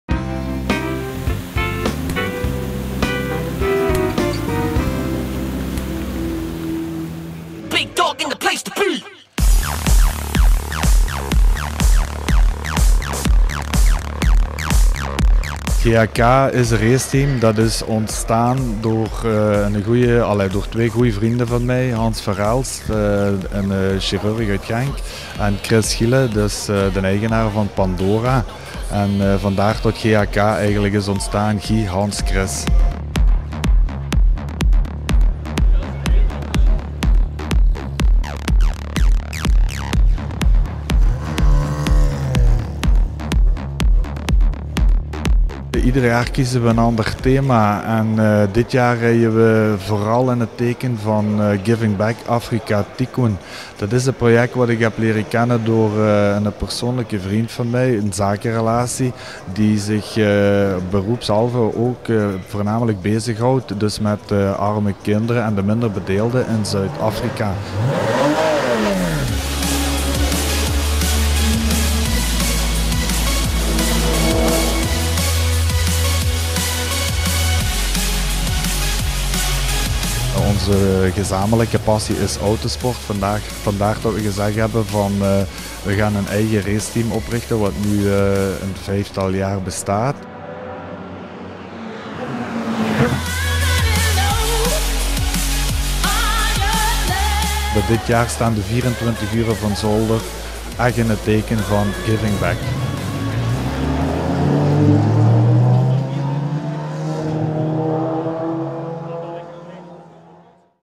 Houthalen-Helchteren: voorstelling nieuwe racewagens GHK Racing
16 juni: nieuwe racewagens, een Radical RXC van Domec Racing voor de 24 Hours of Zolder en een Porsche 991 GT3 Cup van 2014
met quote (vragen aan Esther) 1.5 uur ter plaatse filmen

19 juni :sponsordag (sfeerverslag) 1 uur ter plaatse  filmen,info vragen aan Esther wat te filmen